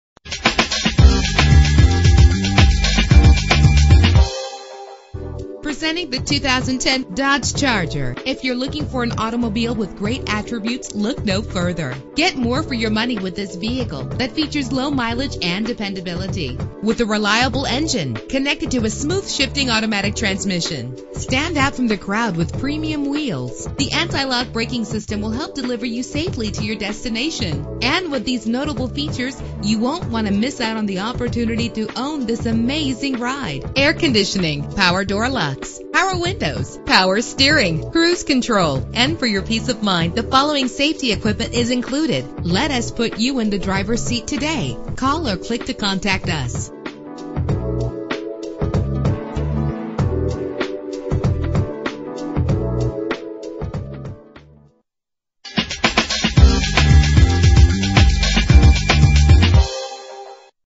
2010 Dodge Charger - Tuscaloosa AL
Year: 2010
Make: Dodge
Model: Charger
Trim: SXT Sedan 4D
Engine: 6 cylinder 3.5 liter ho
Transmission:
Color: Black
Mileage: 19412
Address:
550 Skyland Blvd East